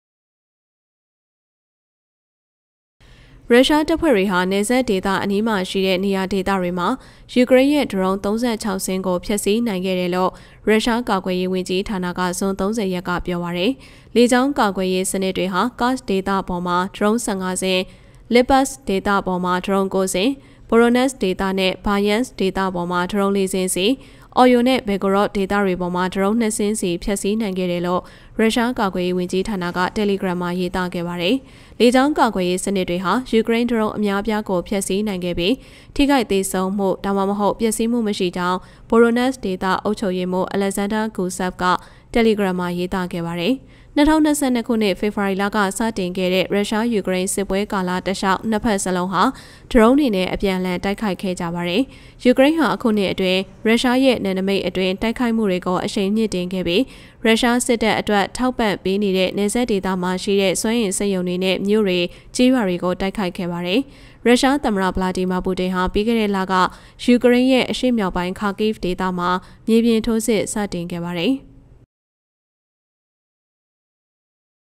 ယူကရိန်း၏ ဒရုန်း ၃၆ စင်းကို ဖျက်ဆီးနိုင်ခဲ့ကြောင်း ရုရှားကာကွယ်ရေးဝန်ကြီး ပြောကြား
ရုရှားတပ်ဖွဲ့ဝင်တွေဟာ နယ်စပ်ဒေသအနီးမှာရှိတဲ့ နေရာဒေသတွေမှာ ယူကရိန်းရဲ့ ဒရုန်း ၃၆ စင်းကို ဖျက်ဆီးနိုင်ခဲ့တယ် လို့ ရုရှားကာကွယ်ရေးဝန်ကြီးဌာနက ဇွန် ၃၀ ရက်မှာ ပြောကြားခဲ့ပါတယ်။